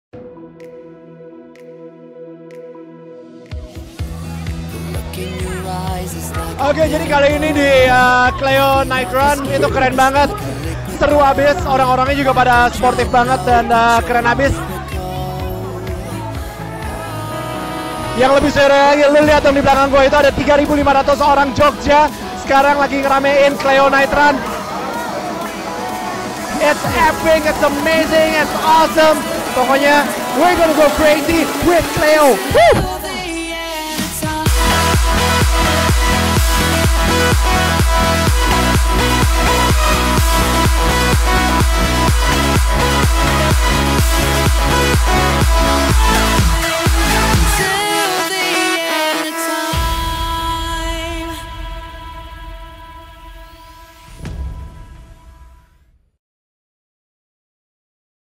Cleo Night Run Fest 2017 Yogjakarta 1 April 2017
Maximum awesomeness night in Jogja bareng @vjdaniel yang bikin makin pecah! walau capek menempuh track yang panjang dan melelahkan kalian tetap luar biasa!!! See you on the next event